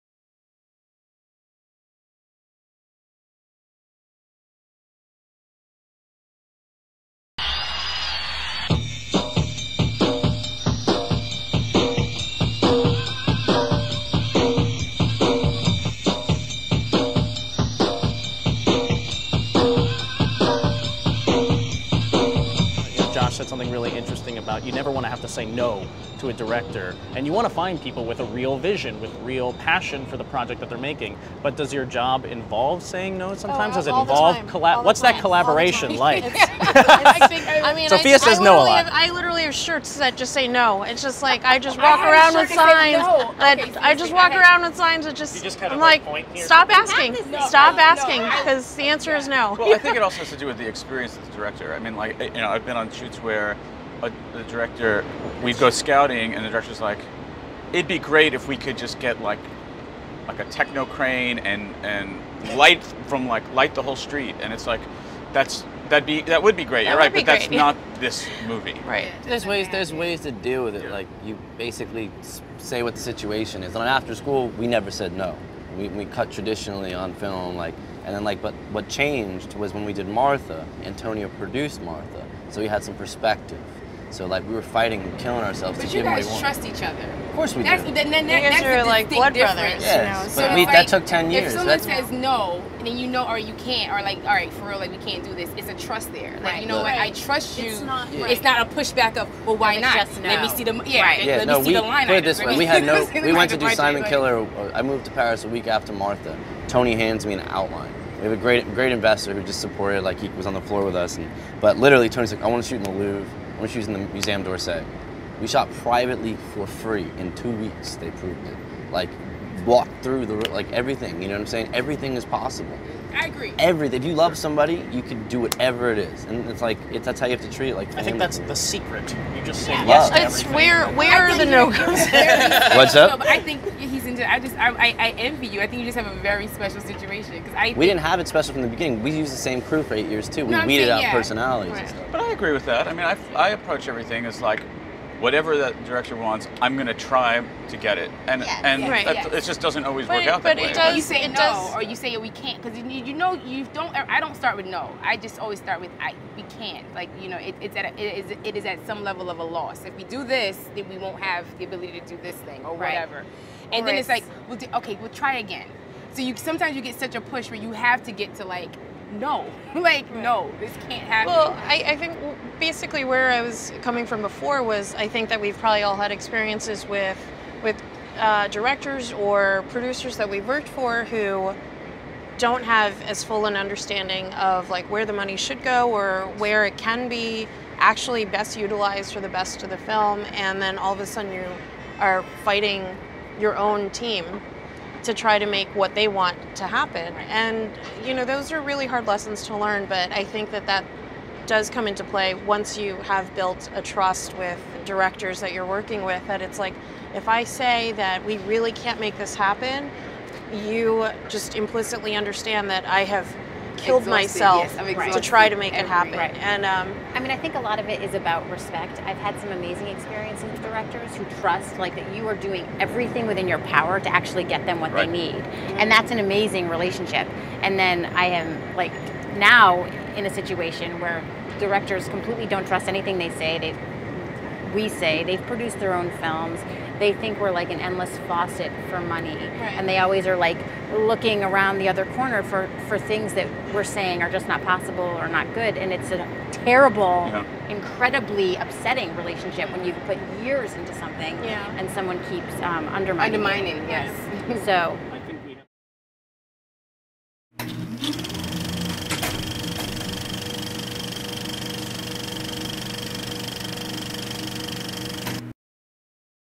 Rambling On... with Producers: The Answer is NO (ROUNDTABLE)
SERIES PREMIERE. Roundtable interview with SOPHIA LIN ("Compliance", "Take Shelter"), JOSH MOND ("Martha Marcy May Marlene", "Simon Killer"), RIVA MARKER ("What Maisie Knew", "The Kids Are Alright"), JARED GOLDMAN "(The Magic of Belle Isle", "The Wackness"), MICHELLE-ANNE SMALL ("Gun Hill Road", "Kiss of Chaos")